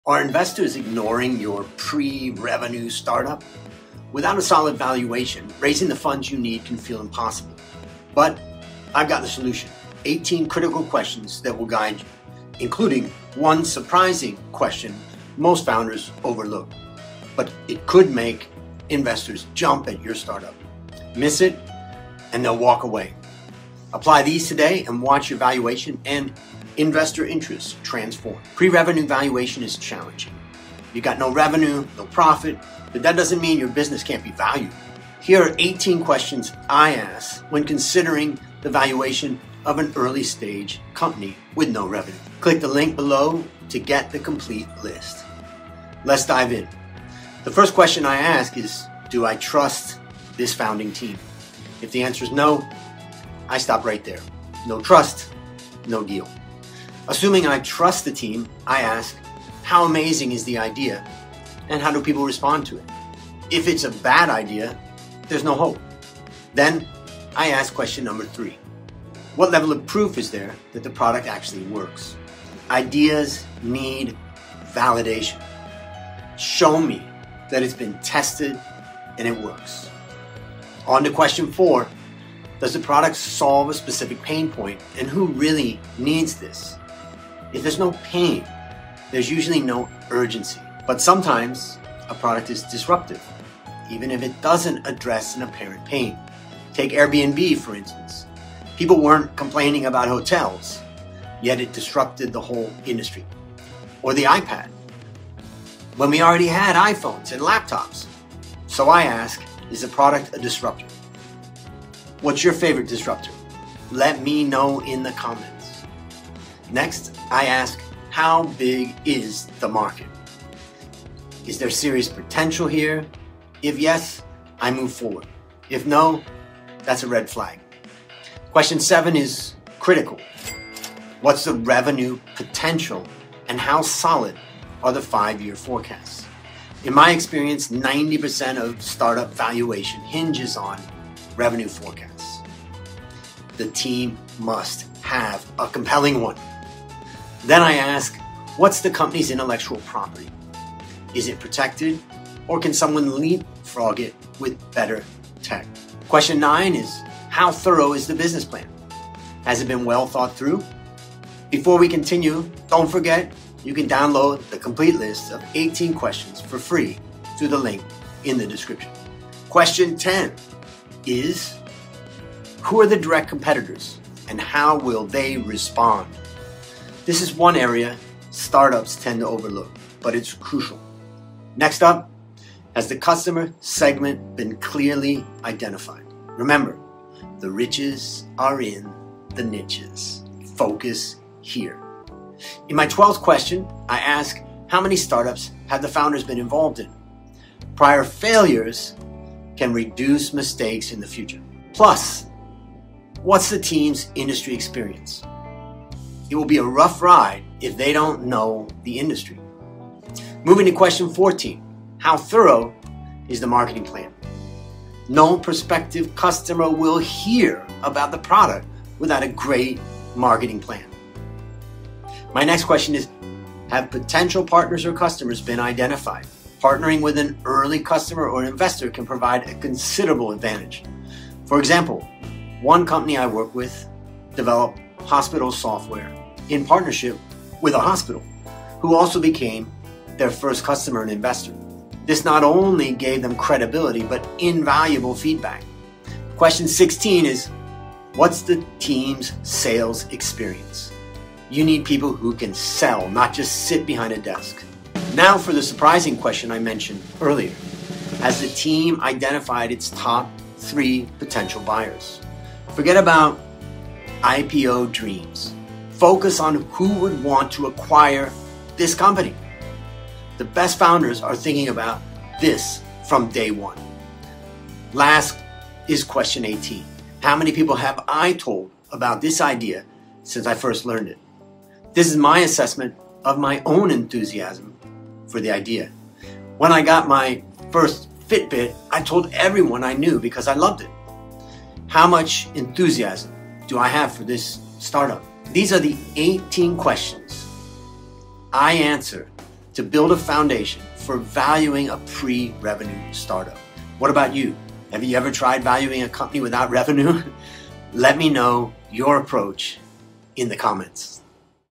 How to raise funds for your Pre-Revenue Startup
Without a solid valuation, you won't be able to raise the funds you need for your startup. So here are 18 questions to help value early-stage companies with no revenue!

I am not your CPA, attorney, or financial advisor and the information in this video shall not be construed as tax, legal, or financial advice from a qualified perspective. Linked items may create a financial benefit.